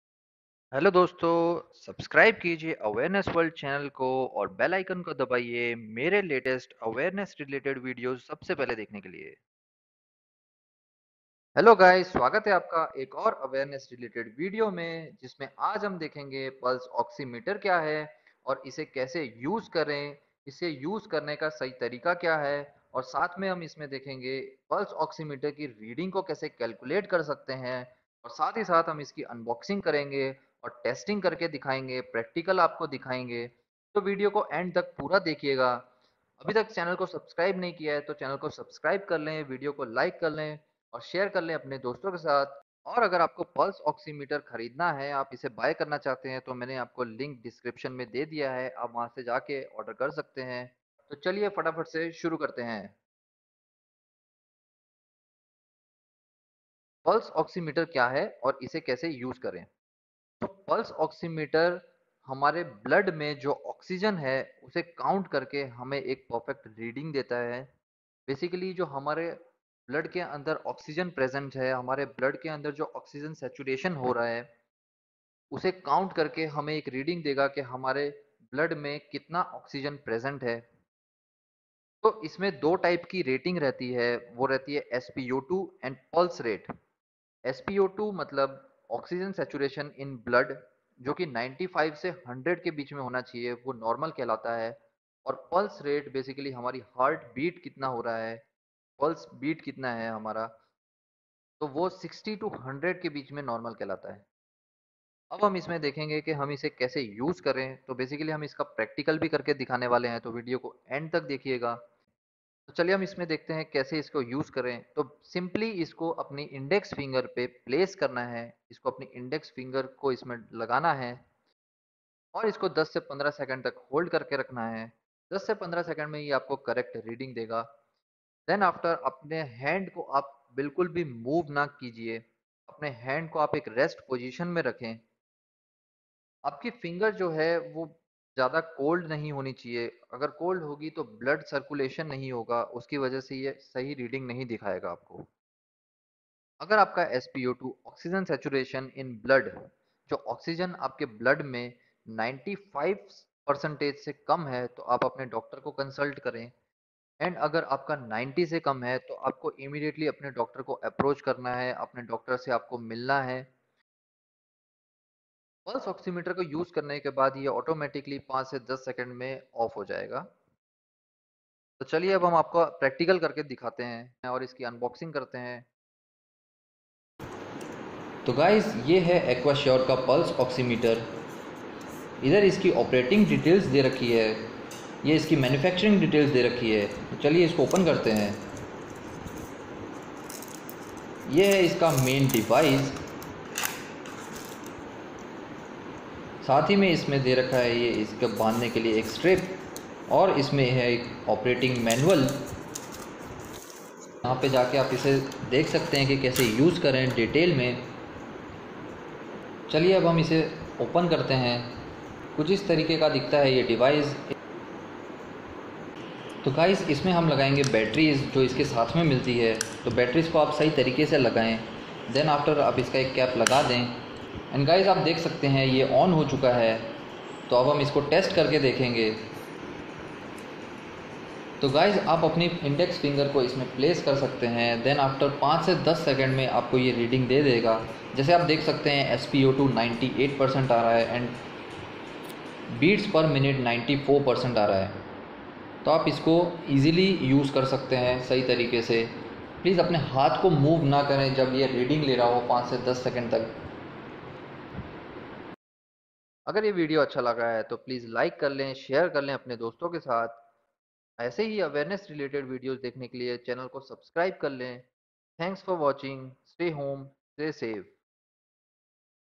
What is Pulse Oximeter ? || Pulse Oximeter कैसे यूज़ करे ? || Unboxing & Testing
In this you will get to know about what is the Pulse Oximeter? How to use the Pulse Oximeter? how to understand the Reading of Pulse Oximeter?
Also unboxing and testing of the Pulse Oximeter and practical to start the pulse oximeter.
If you want to buy Pulse Oximeter, click on the